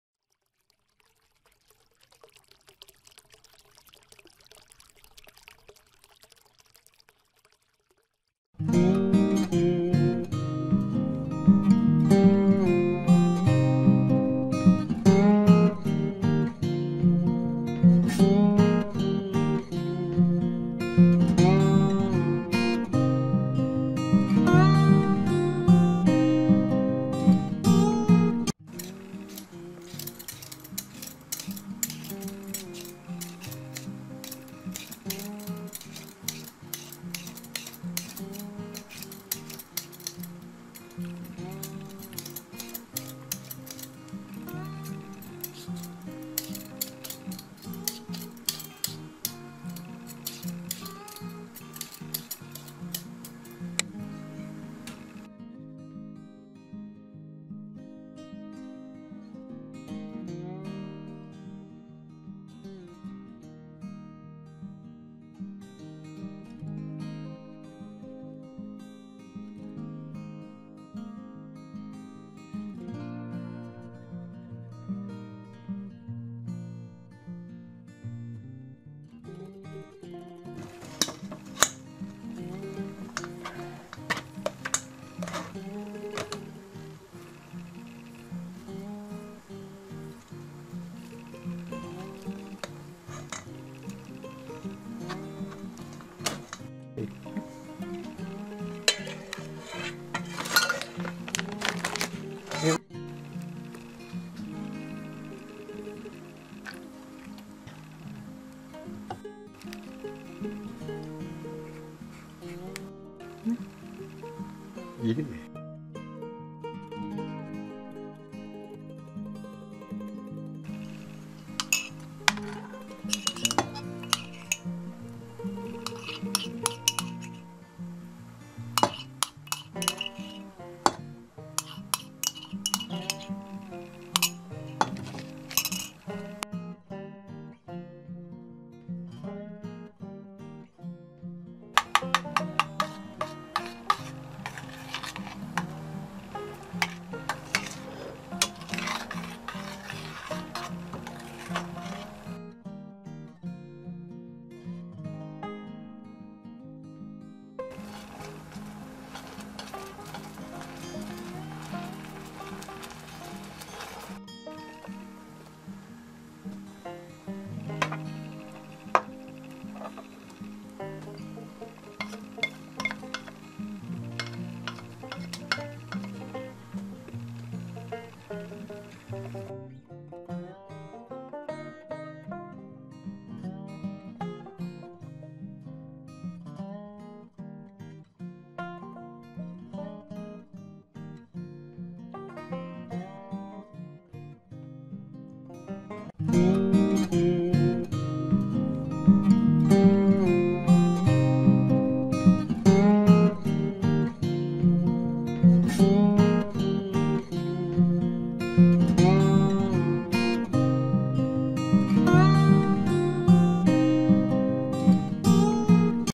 サツマイモ羊羹作り
サツマイモがたくさん採れたので
羊羹を作ります。

設備屋の適当田舎暮らし